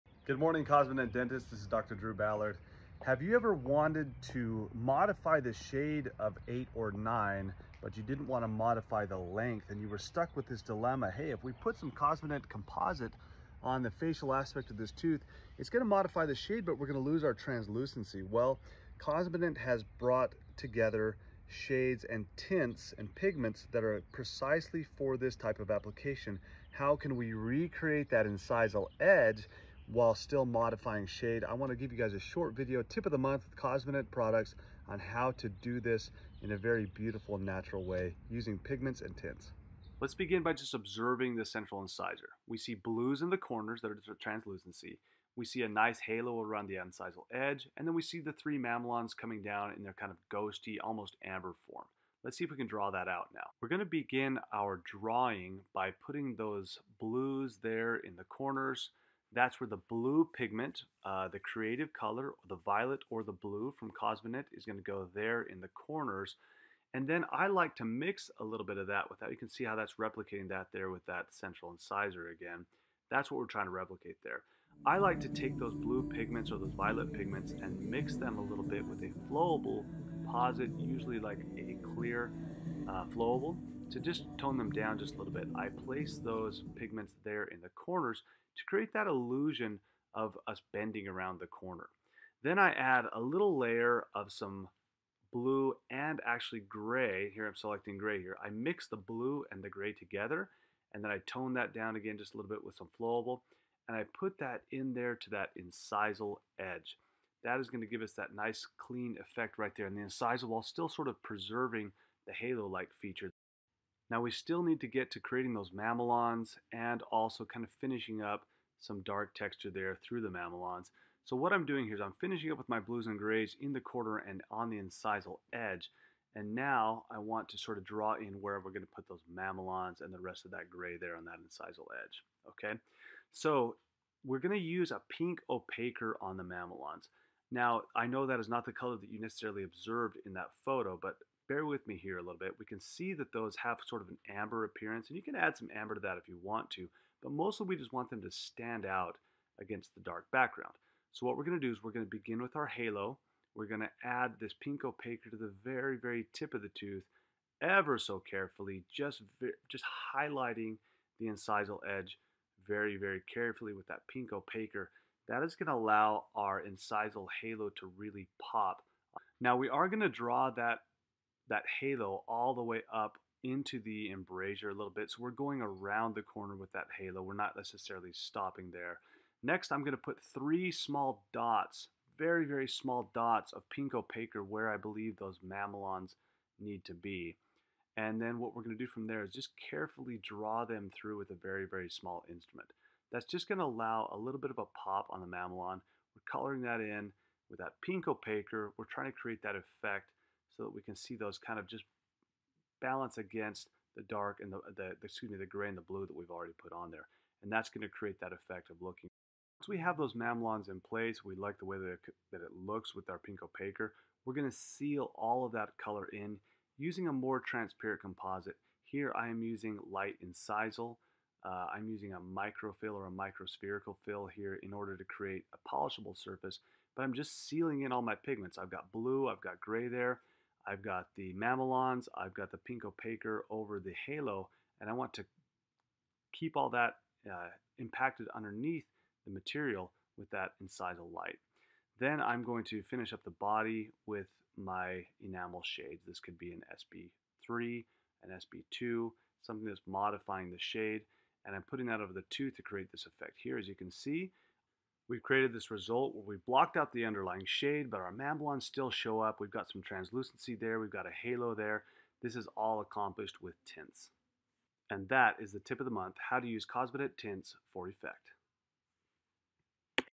Modify the Tooth Shade (without Adding Length) with Tints
🛠️ Products Used in This Video:
Perfect for masking discoloration and building a natural foundation for composite artistry.

Add lifelike details like halos, craze lines, and translucent effects for hyper-realism.

Delivers a natural enamel finish with unmatched polish and esthetics. Ideal for anterior restorations.

00:00 Introduction and tooth anatomy
00:53 Applying the shades and tints
03:27 Application of Microfill and final results

DDr. Drew Ballard, creator of HALO™ veneer technique, shows us how to modify the tooth shade without adding length to the central incisor. Adding straight composite on the facial aspect of this tooth will modify the shade but would reduce the translucency. Using Cosmedent shades, tints, and pigments on the incisal edge with Microfill will adjust the shade but retain the tooth's translucency.

Dr. Ballard starts by explaining the anatomy of the central incisor tooth. He notes the blues (violet) in the corners that are translucent. He points out the halo around the incisal edge, the three mamelons coming down, ghostly – almost amber form.

Dr. Ballard then draws out his plan on his tablet. He puts the Cosmedent Creative Color Violet Tint from Cosmedent in the corners to replicate the blue on each corner of the central incisor. He likes to mix the violet tint with a little clear flowable to tone it down just a bit. Placing the tint will create that illusion of bending around the corner. Dr. Ballard then mixes violet and gray tints to place along the incisal edge, again toning it down with a flowable composite – this preserves the “halo-like” effect.

Dr. Ballard uses Cosmedent Pink Opaque to create the mamelons and complete the halo effect. This will stand out against the dark background. He first draws the Pink Opaque along the very tip of the incisal edge – this will really “pop” the halo.

To finish the mamelons, Dr. Ballard puts three very, very small dots of pink opaque or where they should be. He then carefully draws them through with a very, very small instrument. Again, this will create a “pop” against the darker background.

Dr. Ballard then uses a light incisal shade of Renamel Microfill to seal in all of the tints and opaques and create a polishable surface. He then finishes the restoration with the body enamel—the body shade of Renamel Microfill could be an SB2 or SB3.